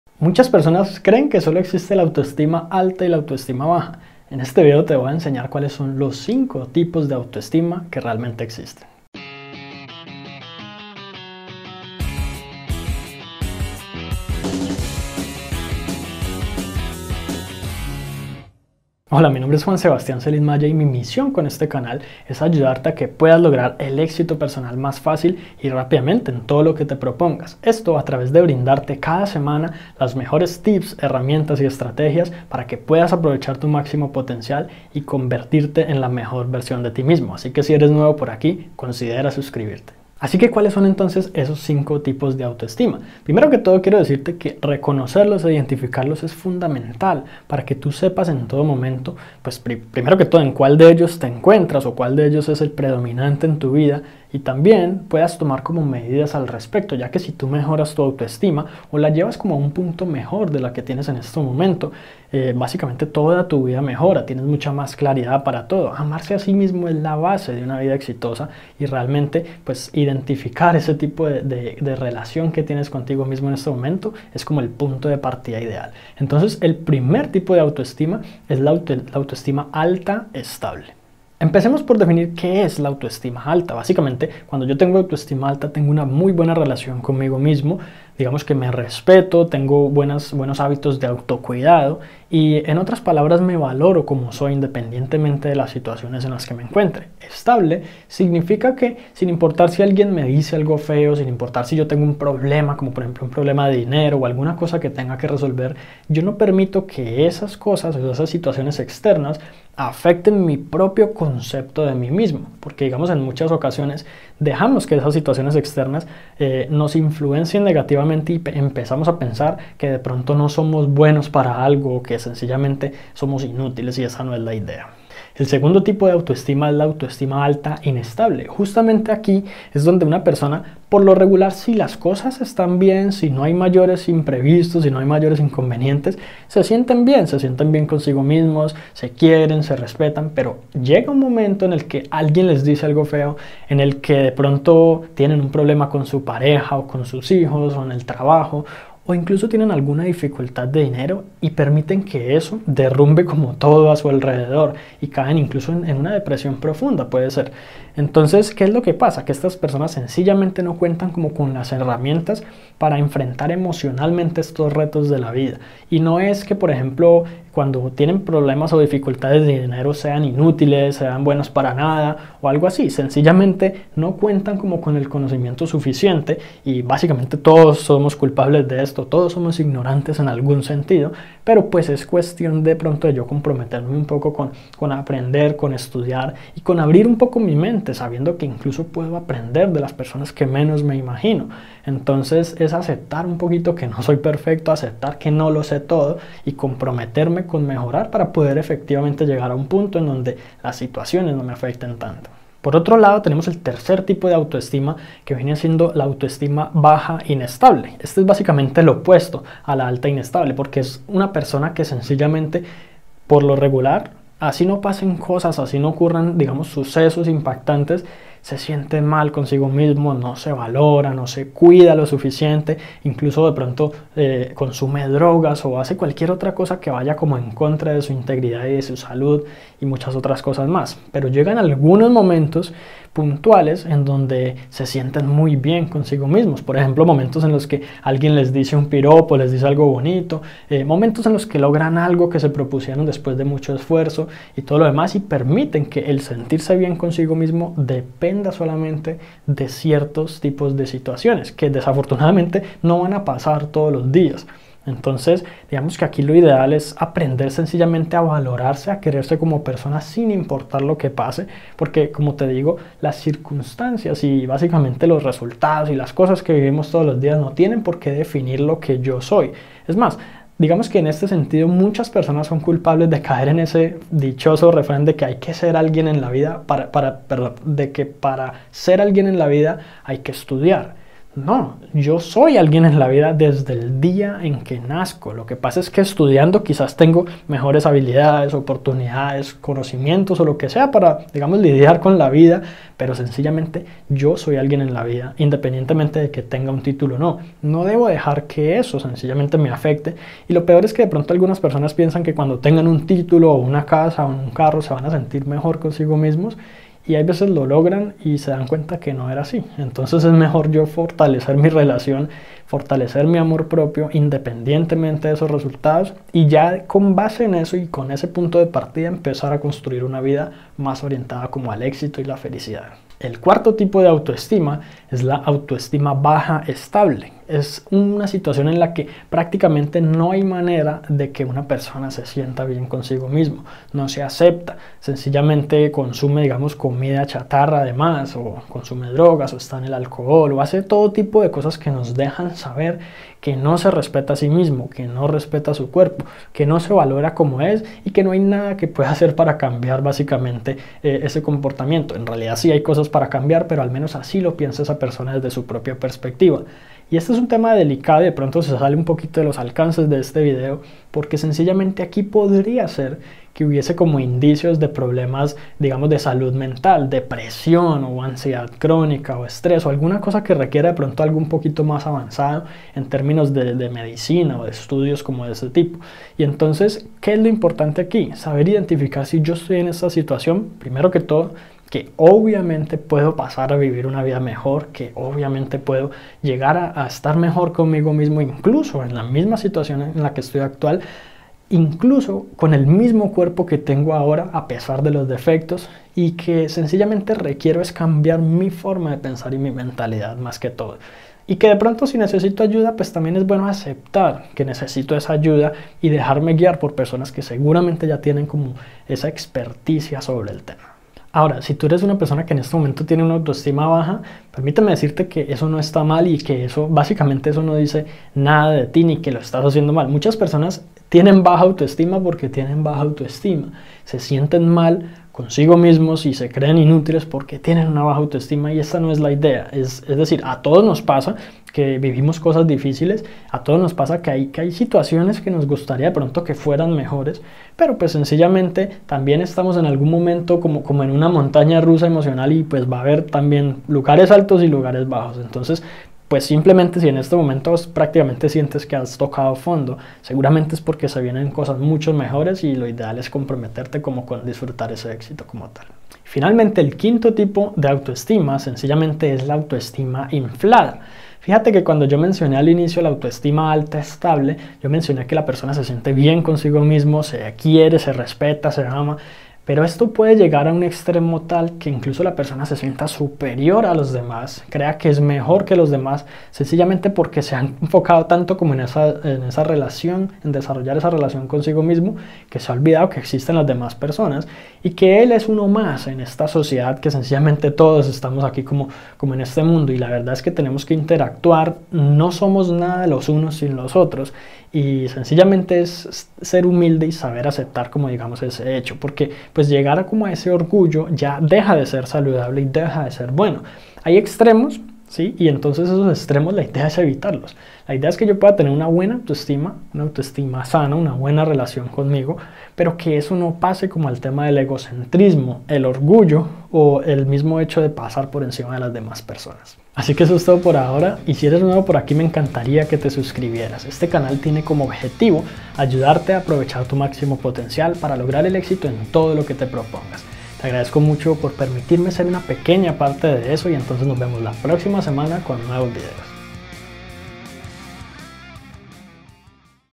Los 5 Tipos De Autoestima - ¿Cuál De Todos Tienes?
Existen 5 tipos de autoestima. ¿Cuál de todos tienes hoy? Descúbrelo ya mismo (te sorprenderá).

Muchas personas creen que sólo existe la autoestima alta y baja. Sin embargo, cuando se trata de definir nuestra relación con nosotros mismos, es mucho más complejo que eso.

Las investigaciones han revelado que en realidad existen 5 clases de autoestima y lo más normal es que tengamos uno de ellos predominante en nuestra vida.

Estas formas de autoestima determinarán cómo te sientes, cómo reaccionas ante las situaciones y qué tan estable es tu salud mental. Descubre cuál es la tuya y si es la ideal o no (y qué hacer en caso de que no).